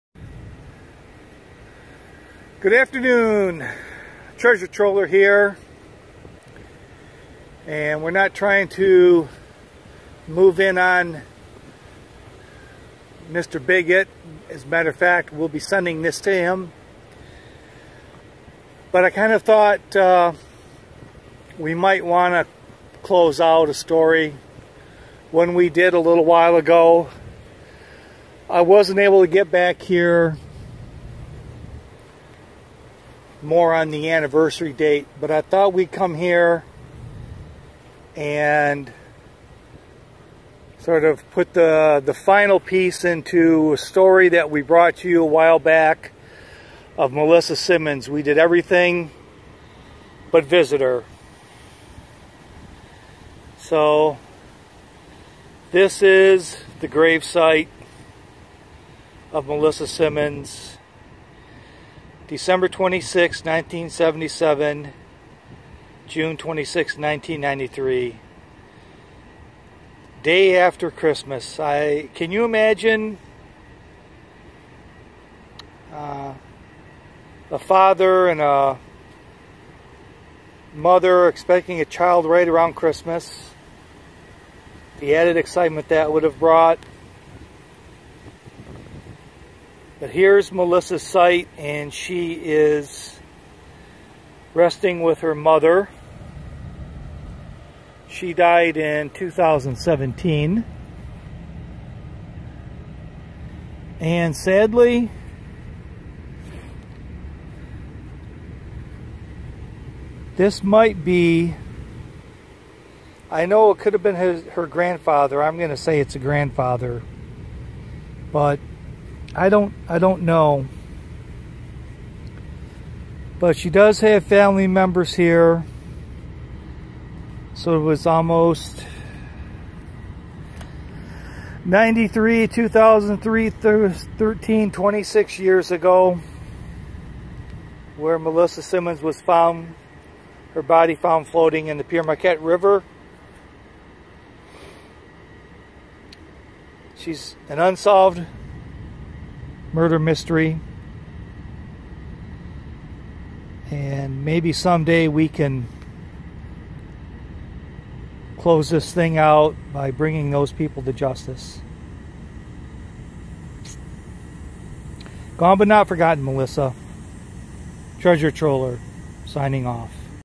The grave of melissa simmons
My buddy treasure troller visits the grave of a young woman who ge did a short video on by the name of Melissa Simmons who was murdered in 1993 her murder remains unsolved to this day.

The link to treasure trollers channel